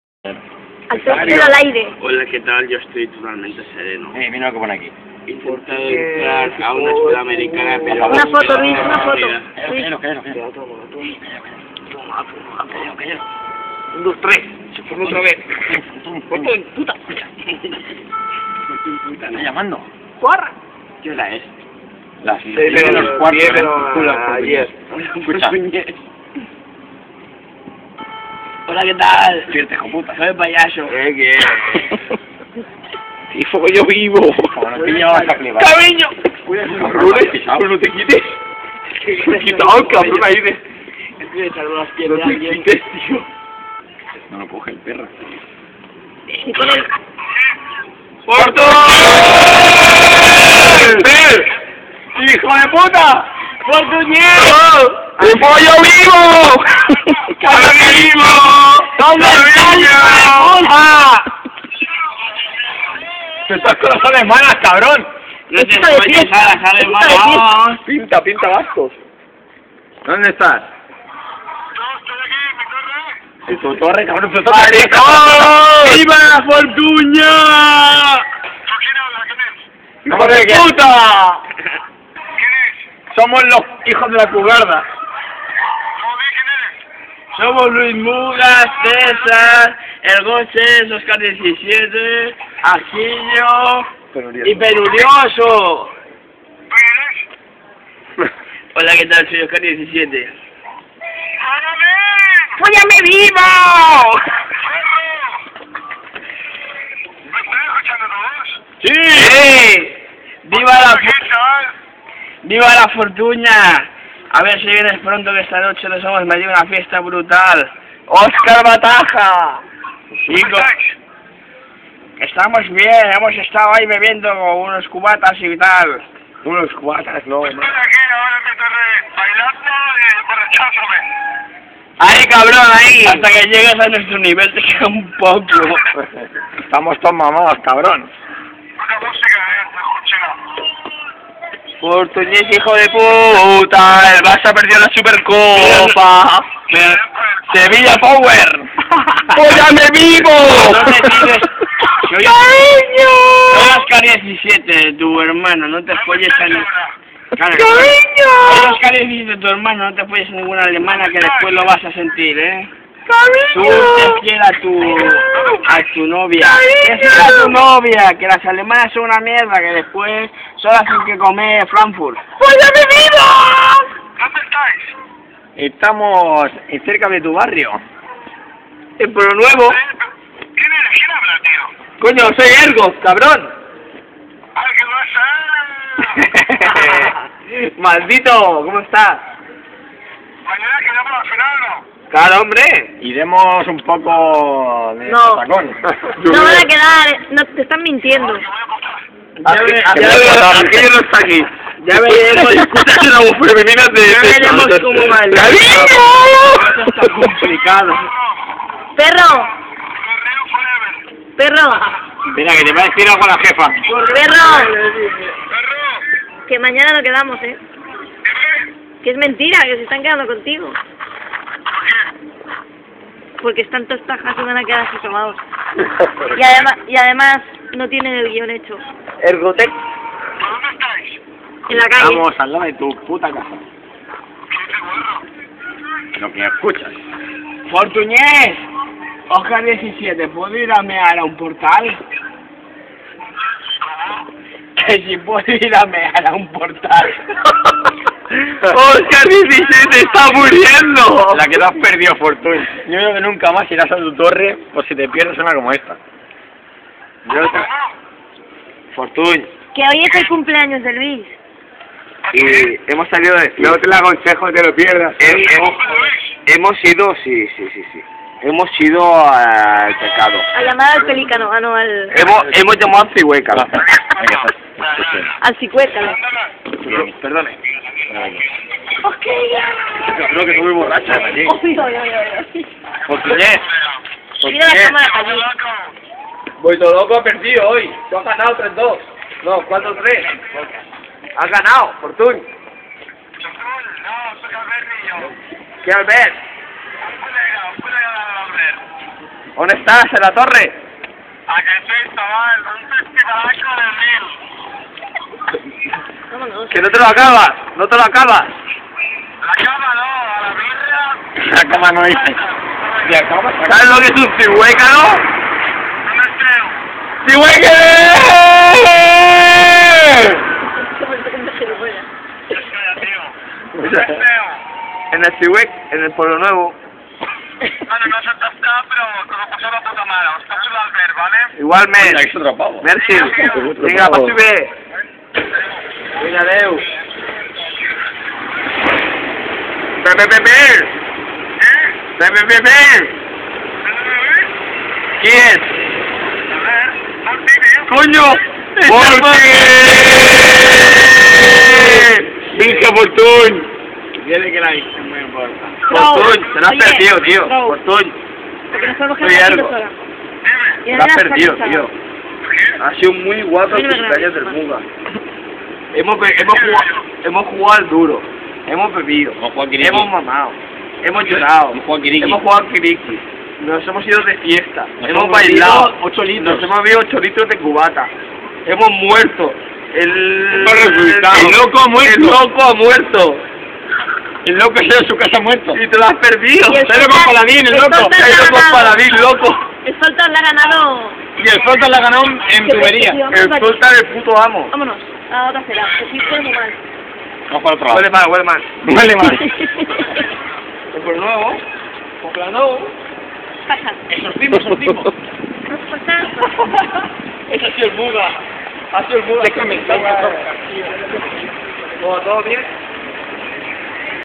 Llamada a Fortuny
Intentamos despertar a Fortuny a las 6 de la mañana pero.